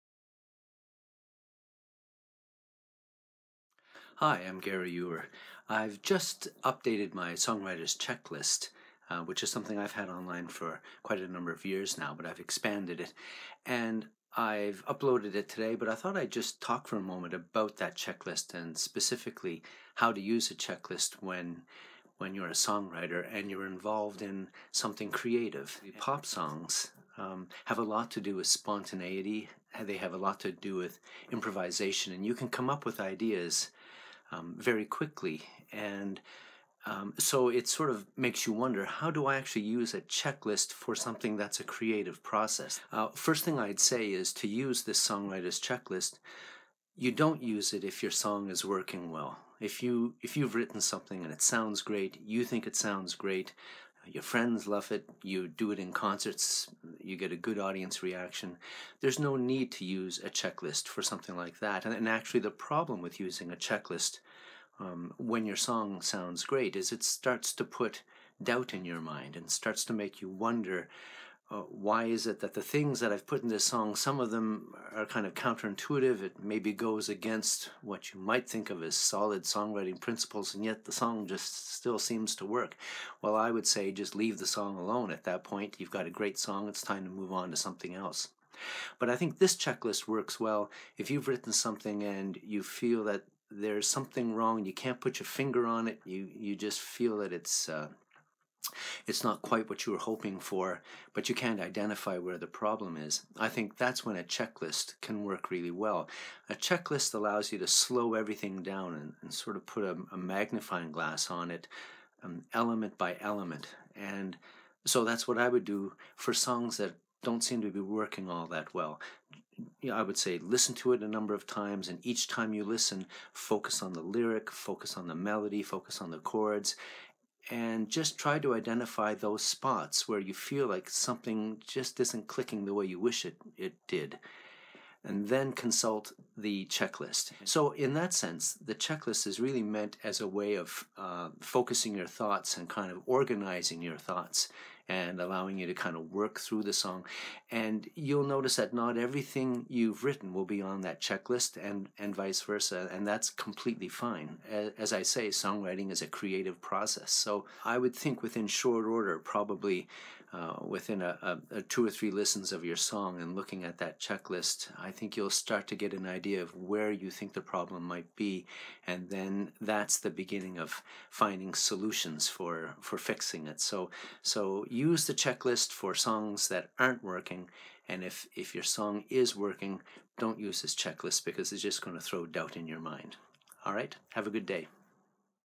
Songwriter's Checklist
A quick description of the free Songwriter's Checklist, written by Gary Ewer.